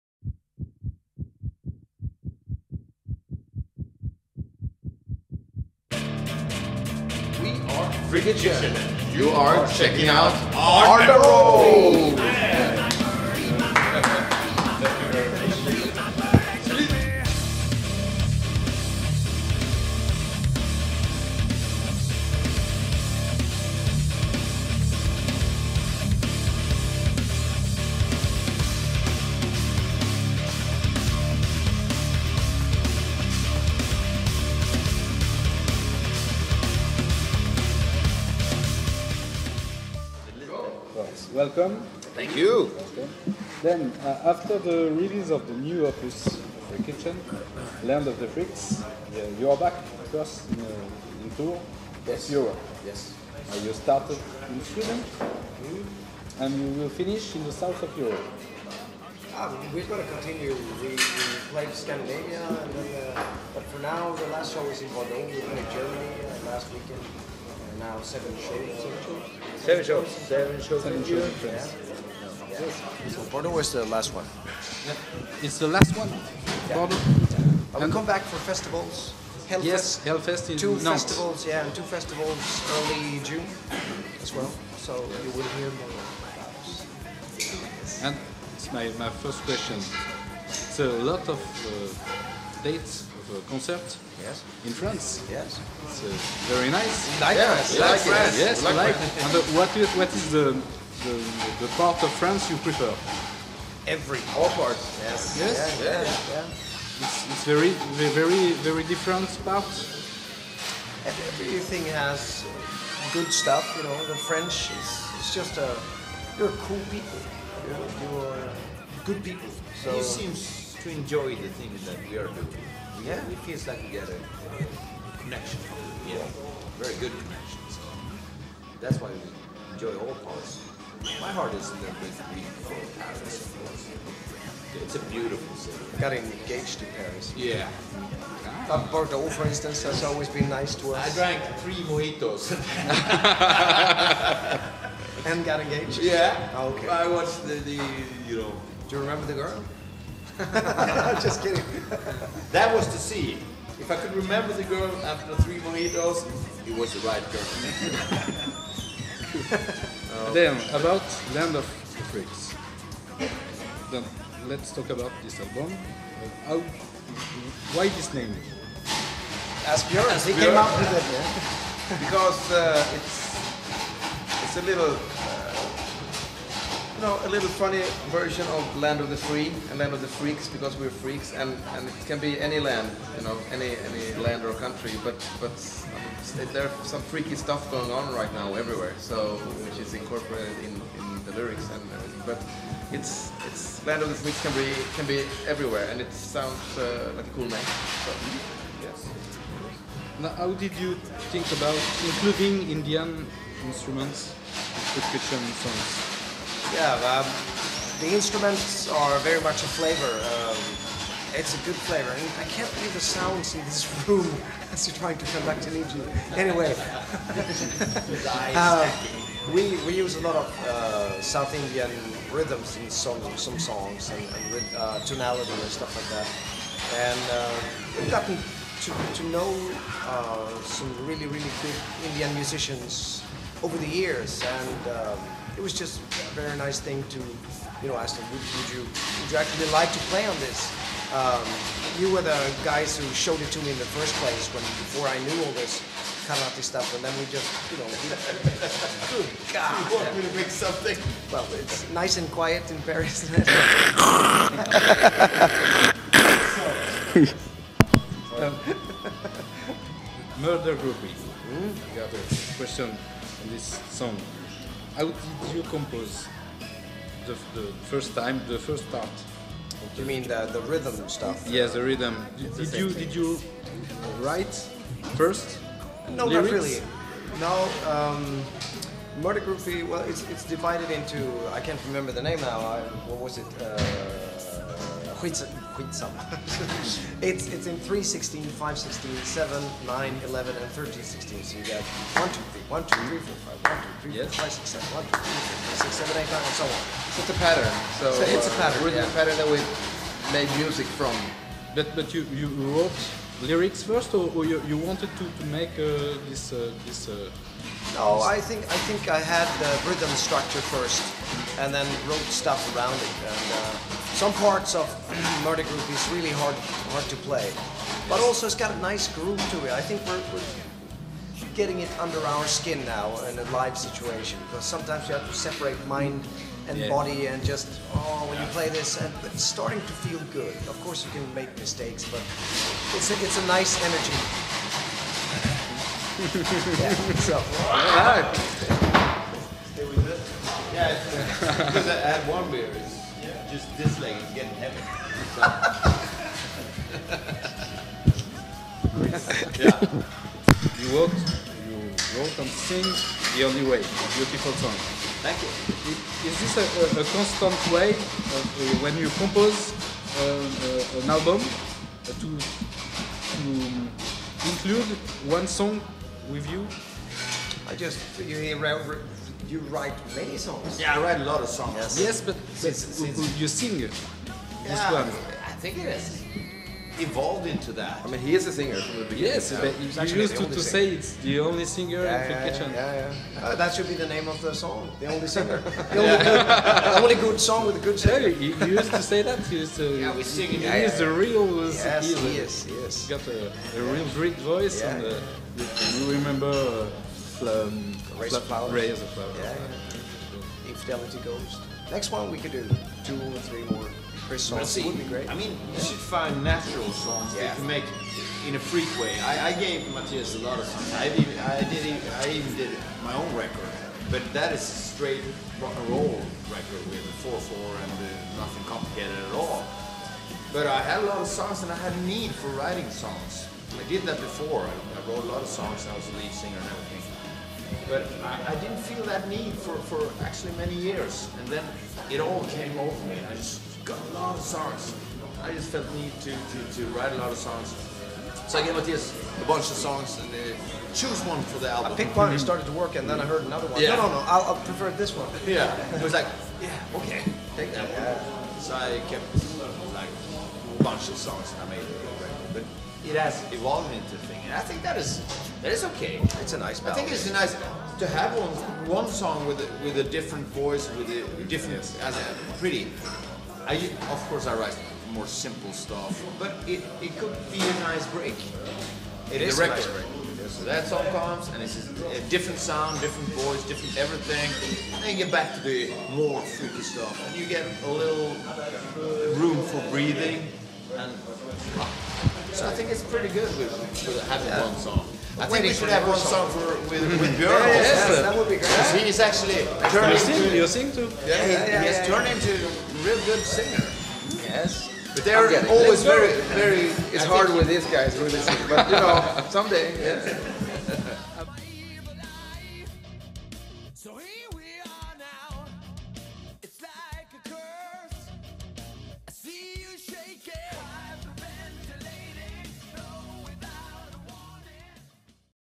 Freak Kitchen 2010 : An interview in Paris, La Maroquinerie, Part 01
During the Tour of the Freaks 2010, Freak Kitchen was interviewed by ArtnRoll Webzine, in Paris just before the show at " La Maroquinerie" in February 08th 2010. Mattias "IA" Eklundh, Björn Fryklund, Christer Örtefors were so fun and cool. This interview is splitted in 2 parts. Enjoy